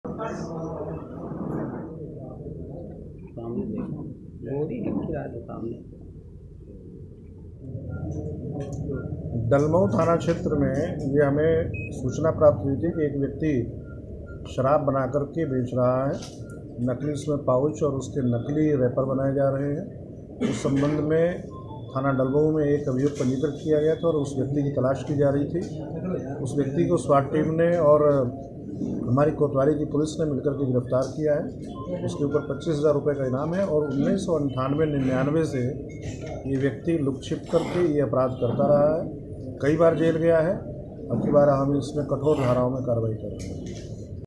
10 किलो गांजे के साथ 25 हजार का इनामी बदमाश गिरफ्तार
रायबरेली: सदर कोतवाली पुलिस को बुधवार को एक बड़ी सफलता हाथ लगी. पुलिस ने चेकिंग के दौरान 25 हजार का इनामी अपराधी को 10 किलो गांजे और एक कार के साथ धर दबोचा. गिरफ्तार आरोपी पर पहले से ही जिले के कई थानों में अपराधिक मुकदमें दर्ज थे.